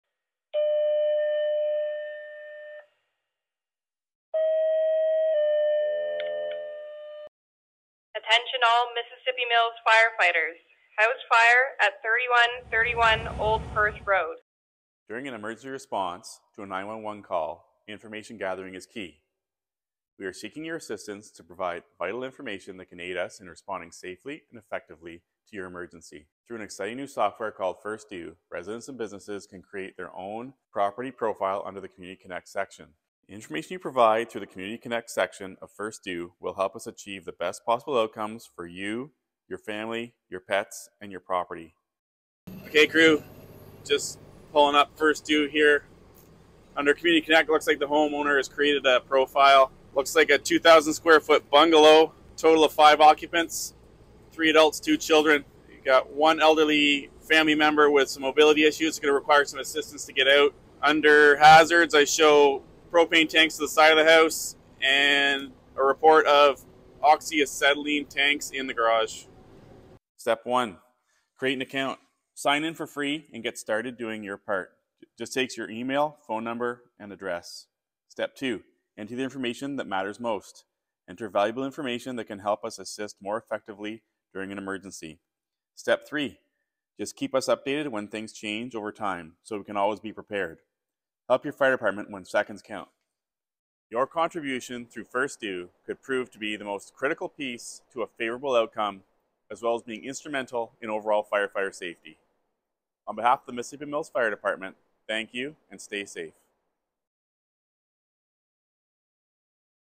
Mississippi Mills Community Connect Promo Video
The Mississippi Mills Fire Department introduces Mississippi Mills Community Connect, a platform designed to increase emergency preparedness and response.

Community Connect is a free, secure and easy to use portal that allows residents and business owners to voluntarily share critical information with the Mississippi Mills Fire Department that will assist emergency responders in the event of an emergency.